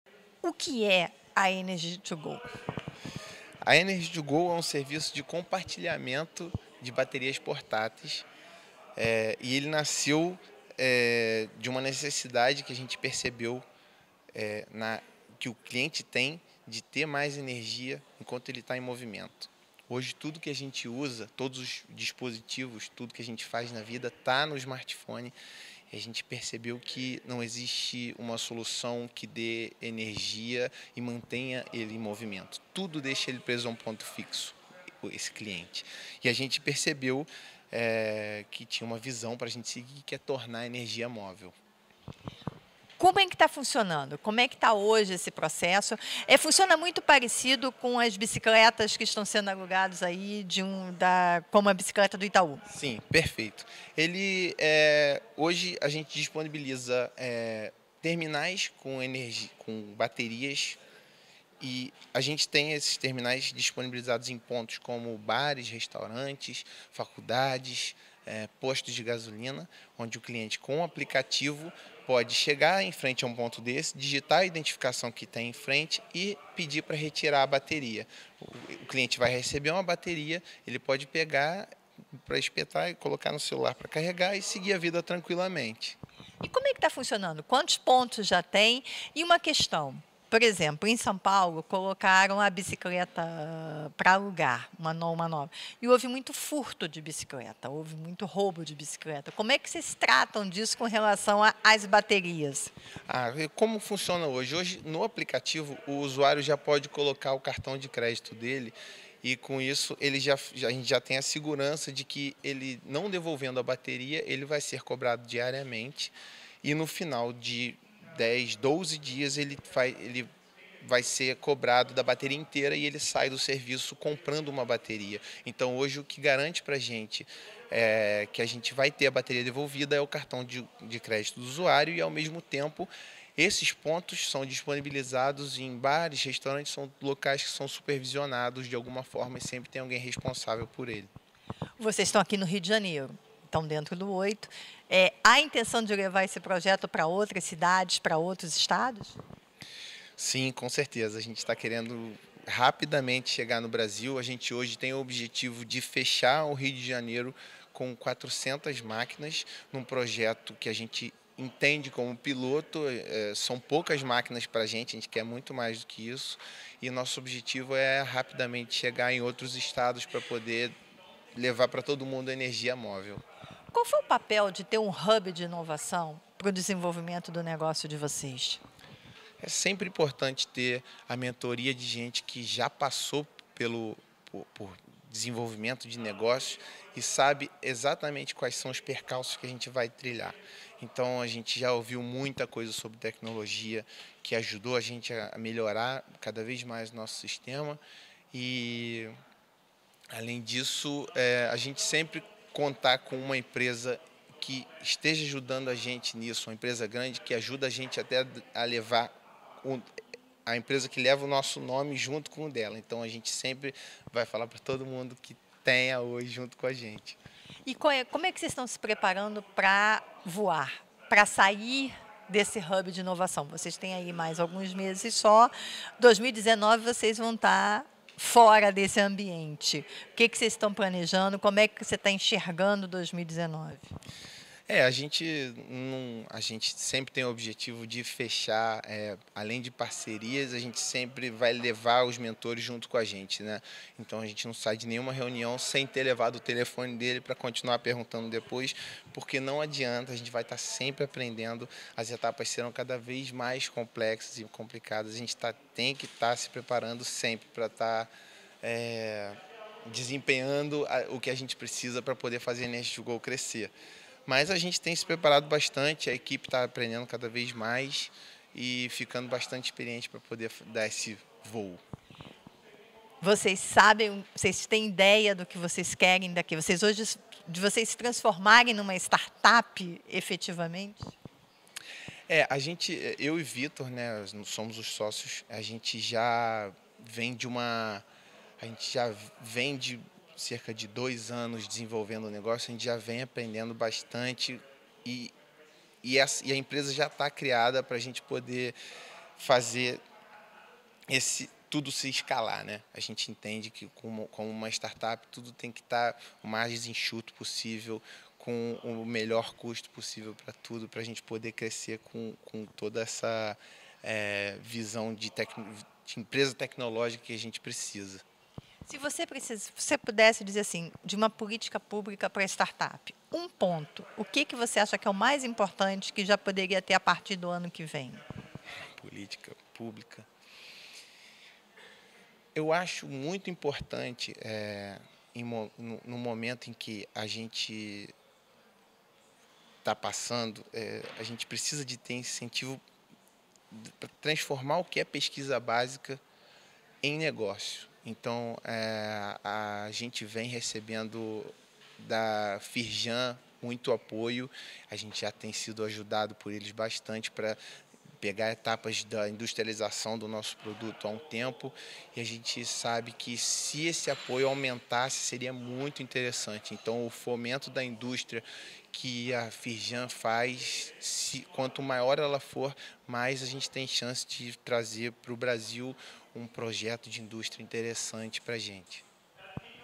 Energy2go planeja ter 400 pontos de energia móvel no Rio de Janeiro
Acelerada pelo Oito, hub de inovação da Oi, a Energy2go tem o plano de chegar a 400 pontos de atuação no Rio de Janeiro, para depois expandir o negócio para outras cidades do Brasil. Na prática, o negócio é voltado para o aluguel de baterias portáteis para recarga de celulares, com o pagamento acontecendo por meio de cartão de crédito.